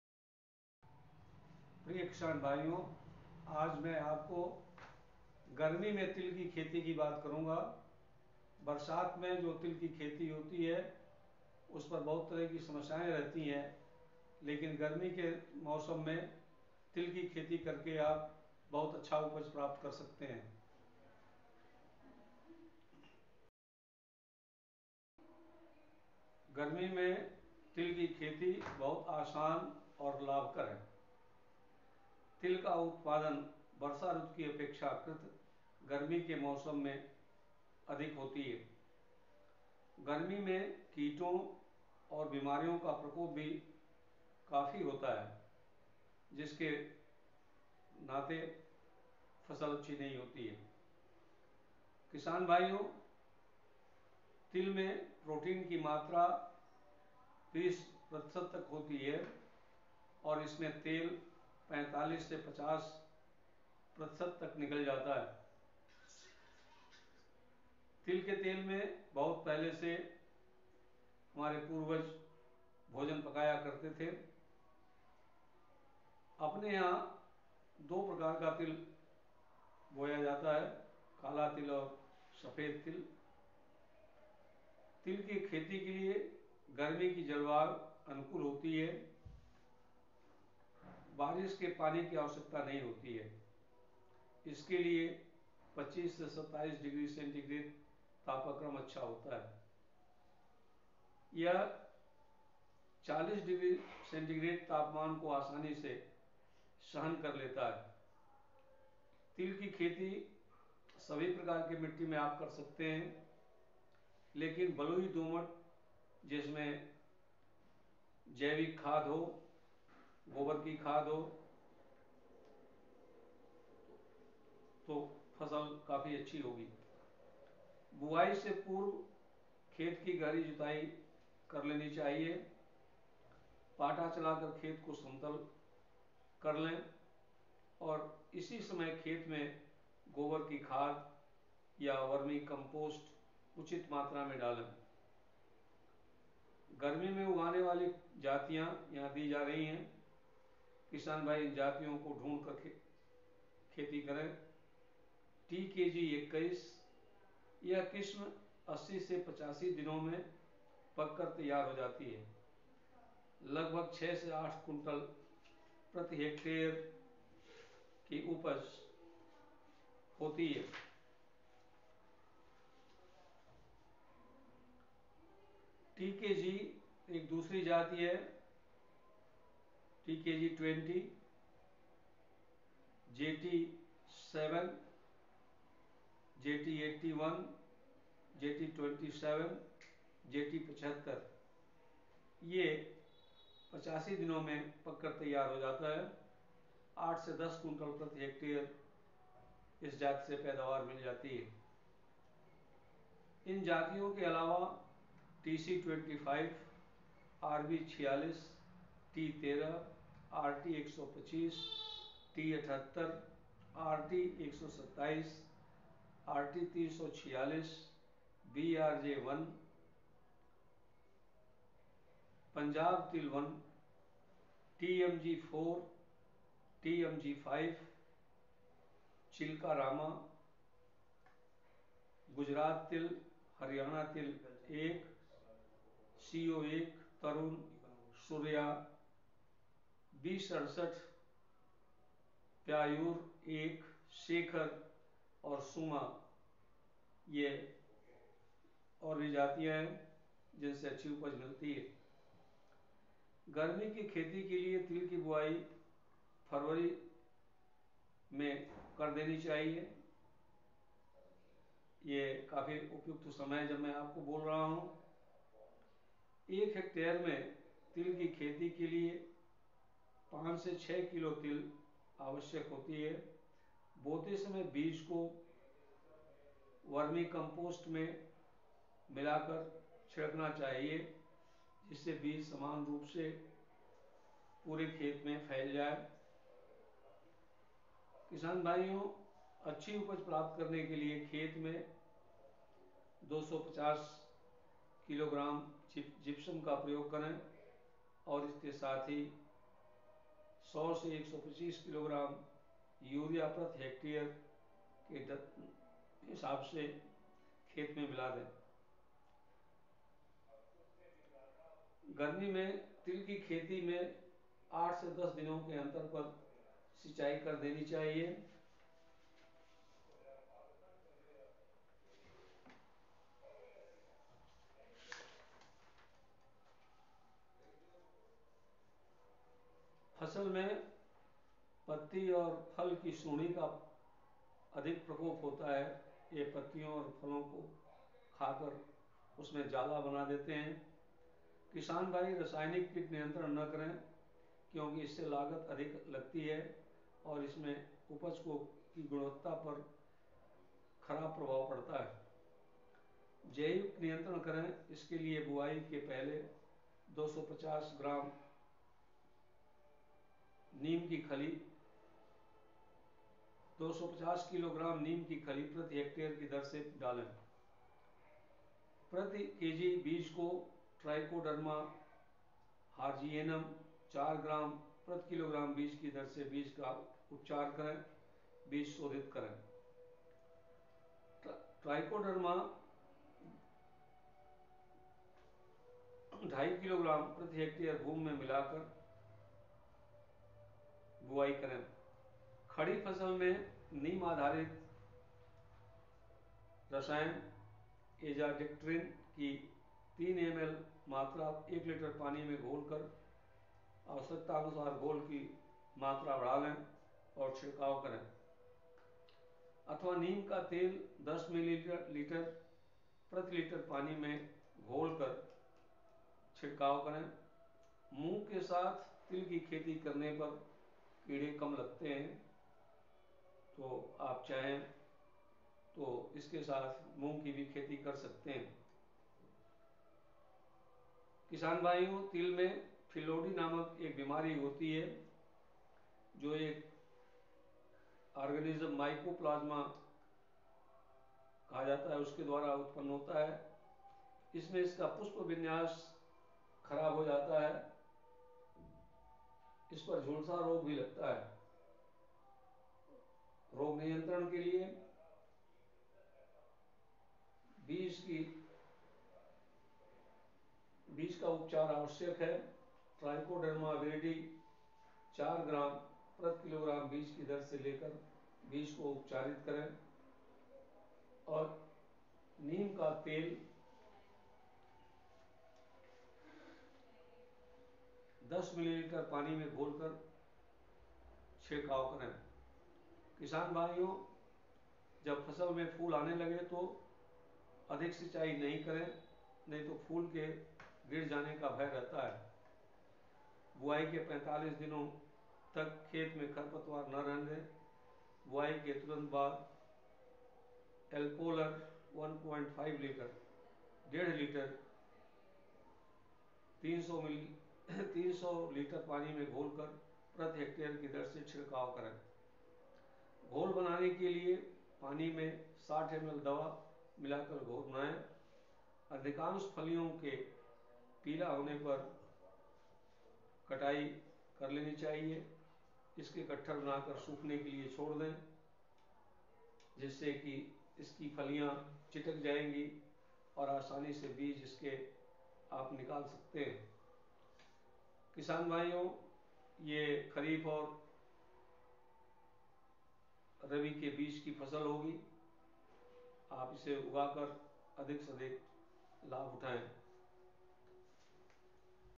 garmi me til ki kheti
aaj ki video mein hum ne Garmi Mein Til ki kheti ke Avan Kiya Hai Samanya Taur par Til ki kheti Barsat Mein ki Jaati Hai jo ki nahin Karan se acchi Nahin Hoti Hai is video ko pura Dekhen aur sab ko share Karen